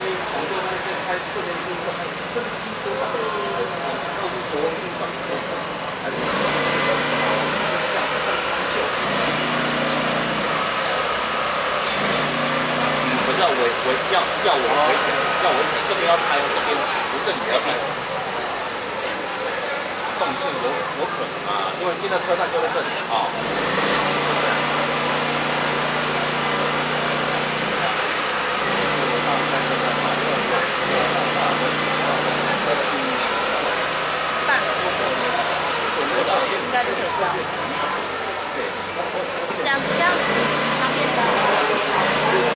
台鐵 高雄地面線路 高雄臨時站 最後一日營業 告別末班車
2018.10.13 台鐵 高雄地面線路 高雄臨時站 最後一日營業 告別末班車